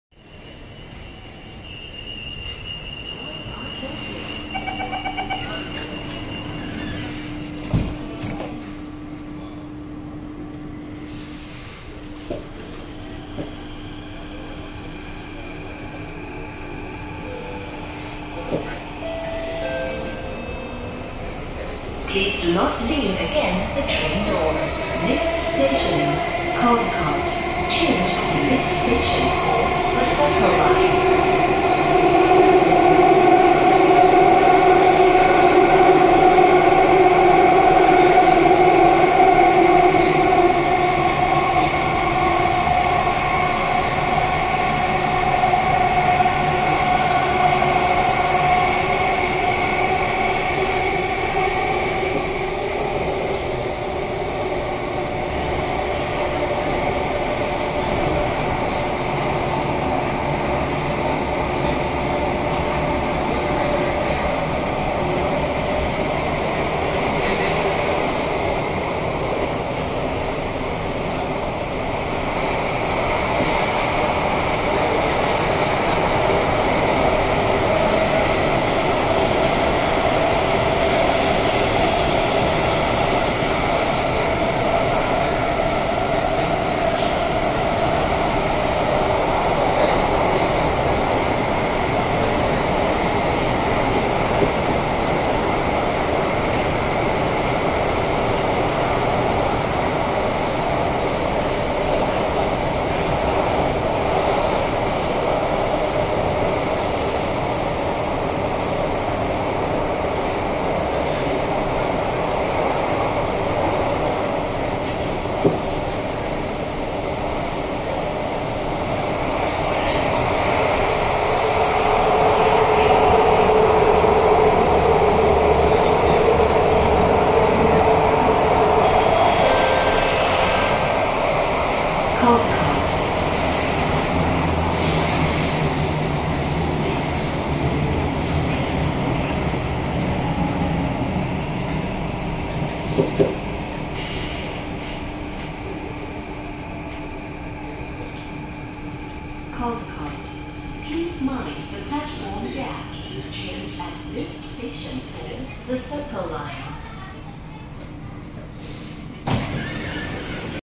[Caldecott Bound - Pre-TEL3] SMRT KSF CT251 [2030] [Upper Thomson → Caldecott] (Termius)
Date filmed: 7 November 2022
Time of recording: 1132hrs
Towards: Caldecott
Train Model: Kawasaki Heavy Industries & CRRC Qingdao CT251
Train Run: 012
Depot of Origin: Mandai Depot
Final Destination Depot: Mandai Depot
Carriage filmed at: 20303 door A1/B1
AM/PM/Full Day: Full Day Service

This video shows a Caldecott Bound, and 6 days (at the point of recording) before TEL3 Opens, and Gardens By The Bay will be the new terminus instead of Caldecott. Note at Caldecott, the RATiS also says No Train Service. That will soon change in the future... Also the DRMD has already been updated to feature the TEL3 stations together as part of the integration testing with TEL1 & 2 stations.